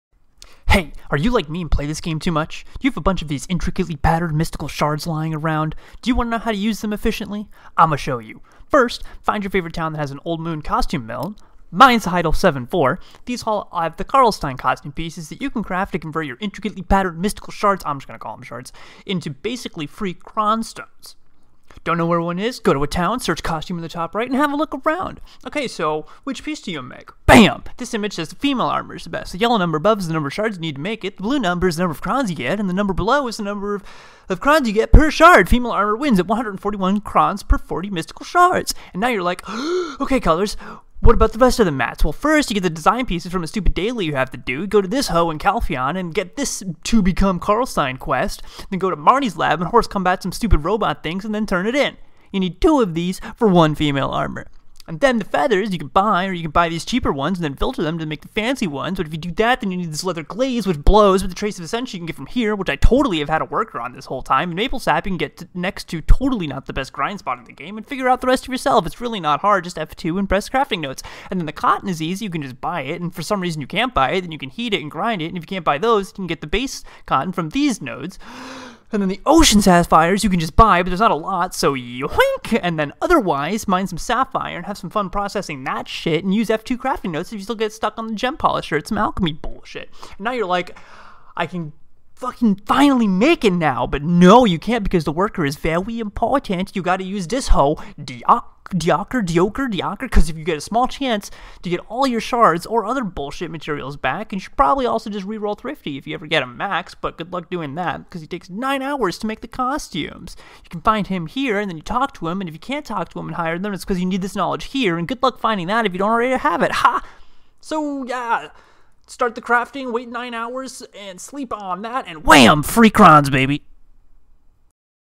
MYSTICAL SHARDS TO FREE CRONS GUIDE
HI ITS MY DUMB QUICK GUIDE TO TURNING YOUR 3 ADJECTIVE INTRICATELY PATTEREND MYSTICAL SHARDS INTO FREE CRONS STONES